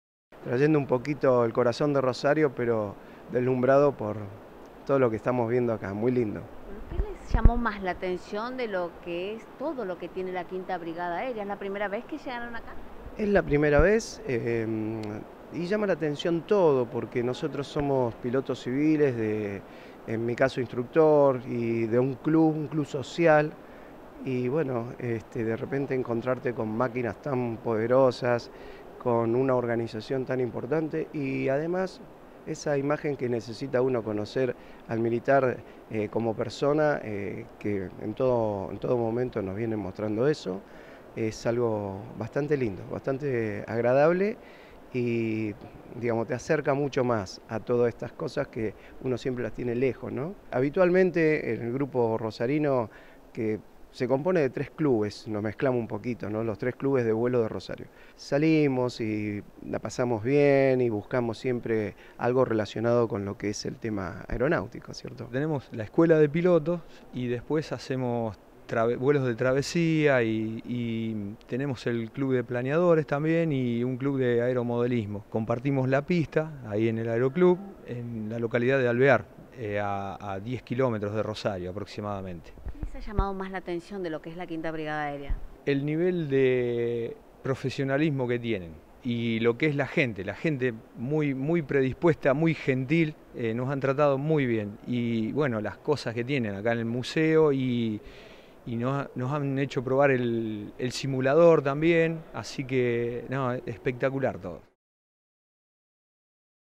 Visita Al Predio De La V Brigada Aérea
✅ Cuatro pilotos del aeroclub Rosarino recorrieron el predio de la V Brigada Aérea de Villa Reynolds, invitados por la Fuerza Aérea. Destacaron sus instalaciones y las diferentes dependencias.